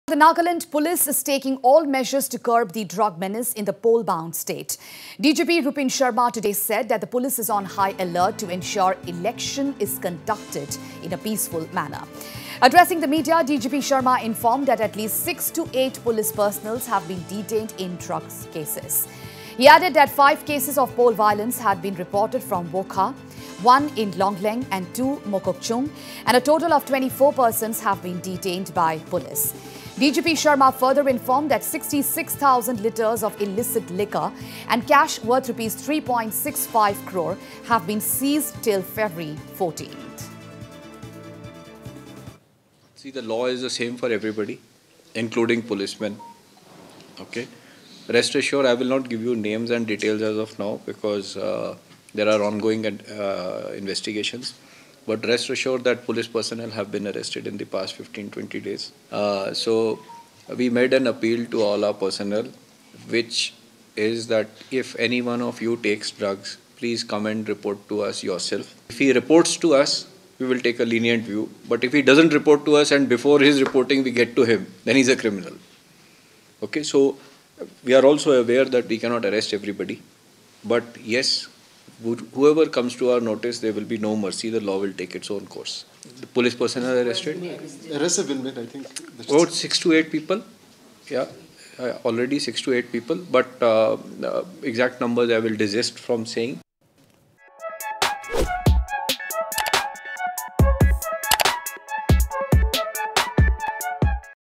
Nagaland polls: Police on high alert to ensure peaceful election
🟦🟥 𝗡𝗢𝗥𝗧𝗛𝗘𝗔𝗦𝗧 𝗟𝗜𝗩𝗘 | 𝗡𝗔𝗚𝗔𝗟𝗔𝗡𝗗 𝗣𝗢𝗟𝗟𝗦
Nagaland police is taking all measures to curb the drug menace in the poll bound state. DGP Rupin Sharma today said that the police is on high alert to ensure election is conducted in a peaceful manner.
Addressing the media, DGP Sharma informed that at least 6 to 8 police personnels have been detained in drugs cases. He added that 5 cases of poll violence had been reported from Wokha, 1 in Longleng, and 2 Mokokchung, and a total of 24 persons have been detained by police.
DGP Sharma further informed that 66,000 litres of illicit liquor, and cash worth Rs.3.65 crore have been seized till February 14